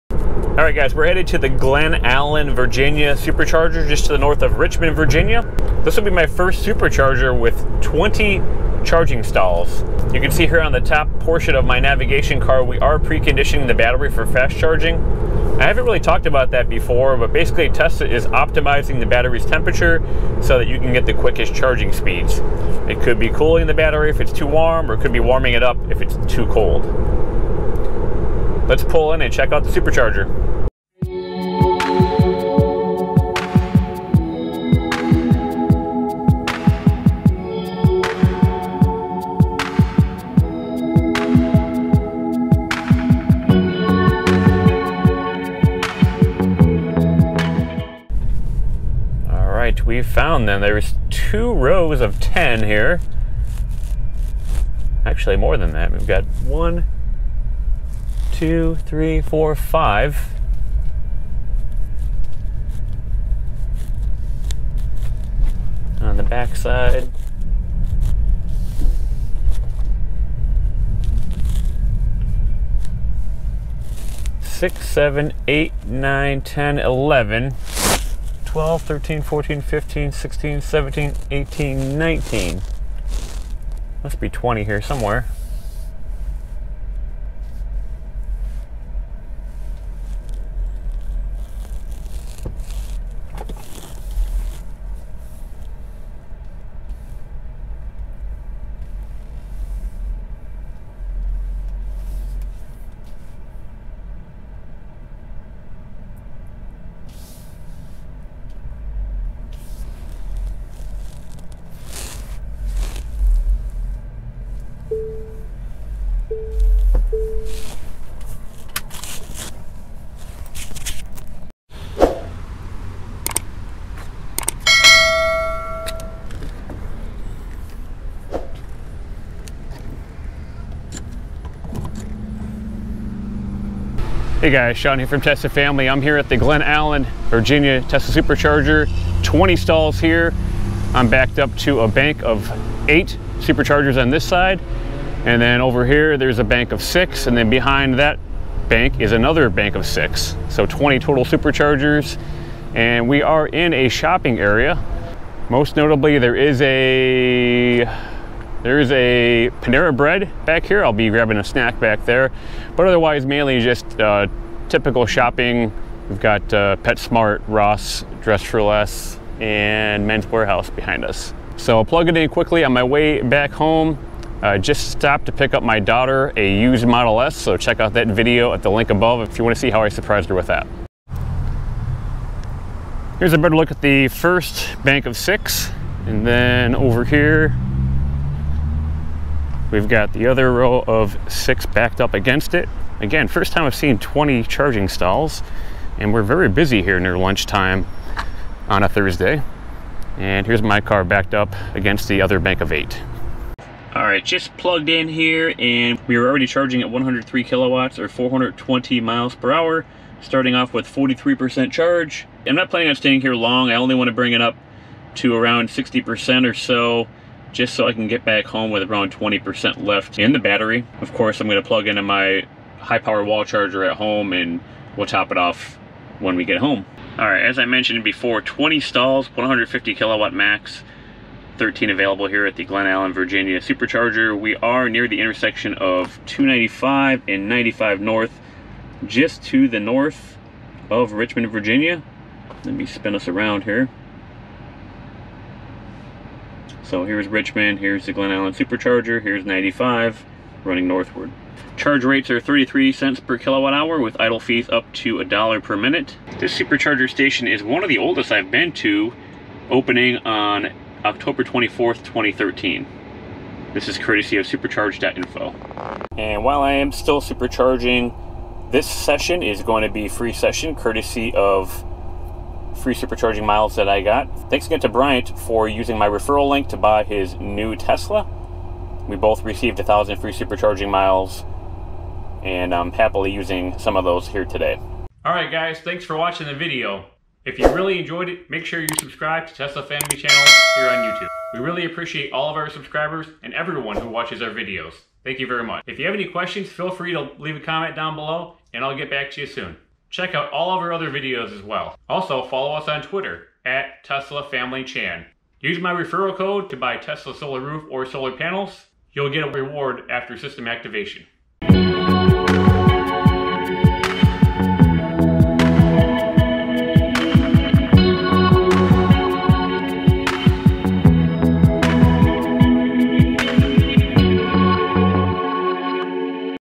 Glen Allen, Virginia / I-95 Tesla Supercharging | Preconditioning battery for fast charging?
Check out the Glen Allen, Virginia Tesla Supercharger north of Richmond on I-95. Twenty V2 charging stalls. I explain battery preconditioning and show you charge rate, cost, and surrounding amenities.